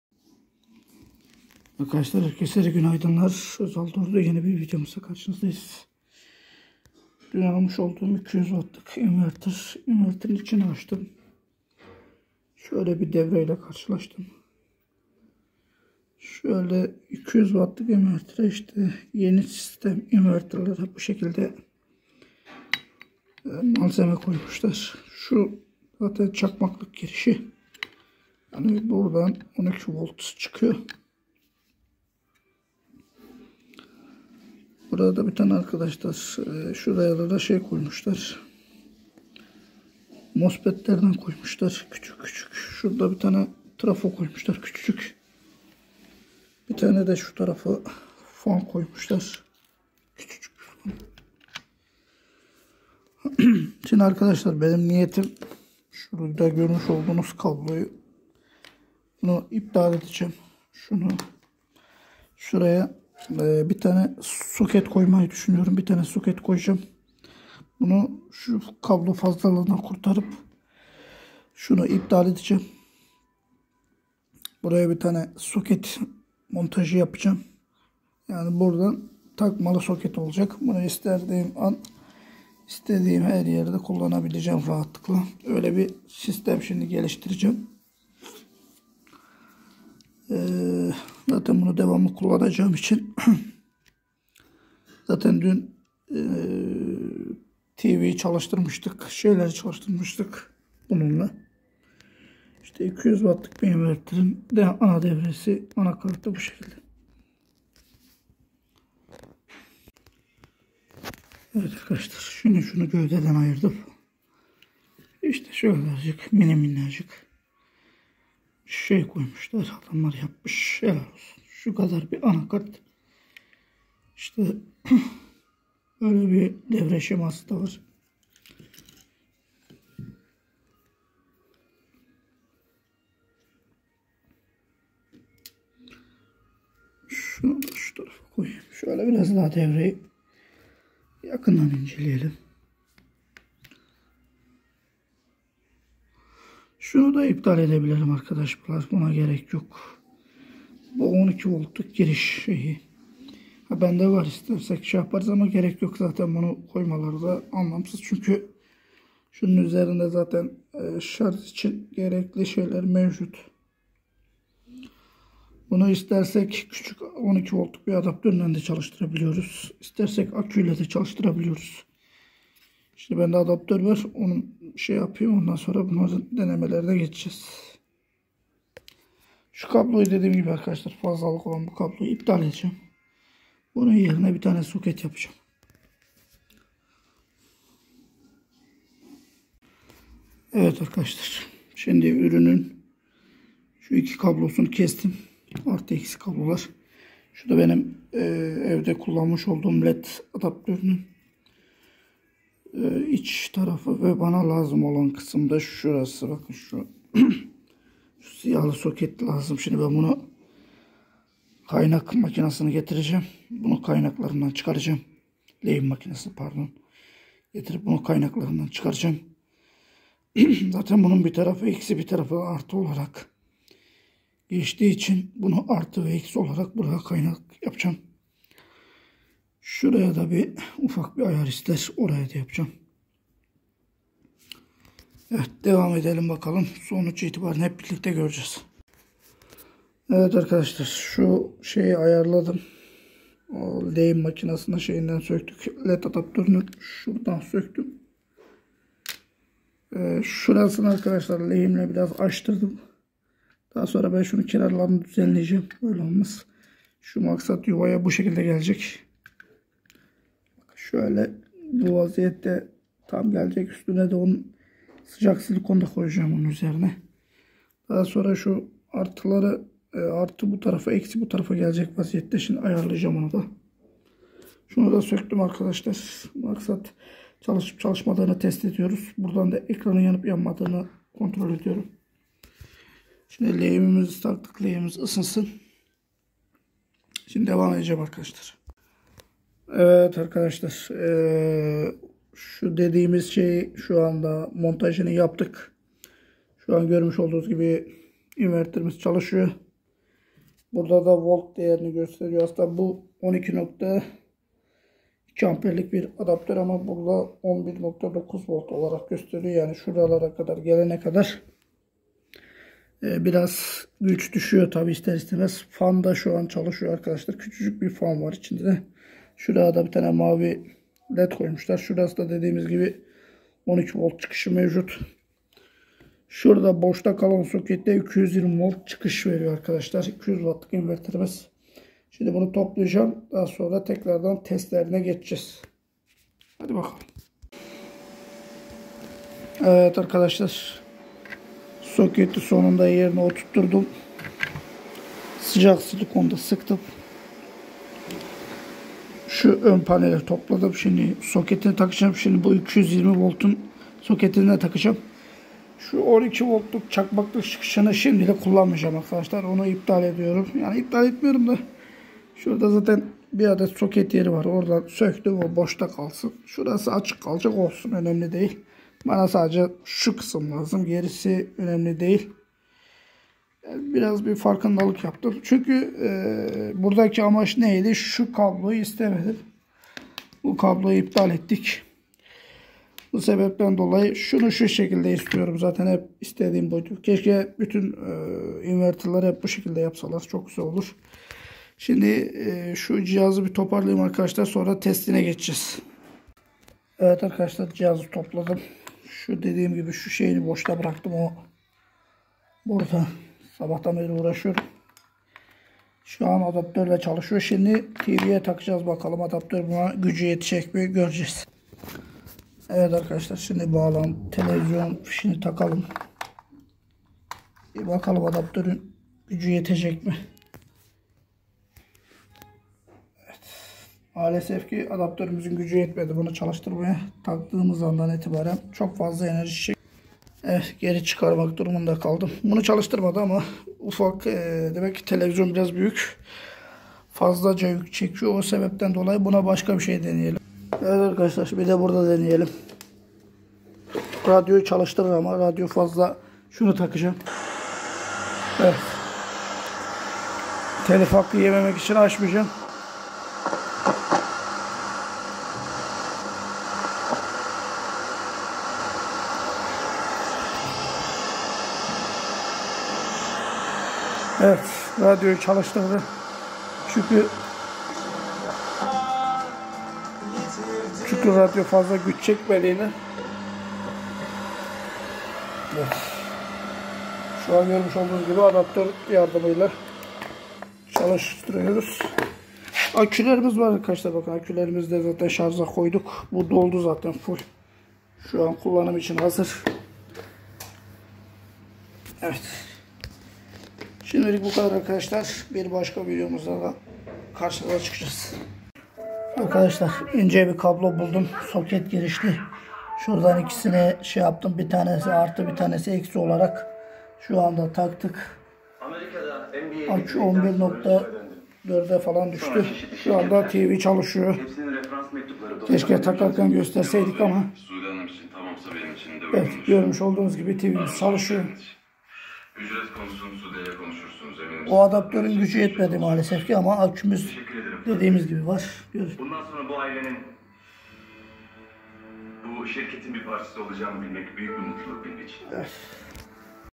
12 volt 200 watlık inverter
kabolsunu kesip iptal ettim  yerine soket girişi taktım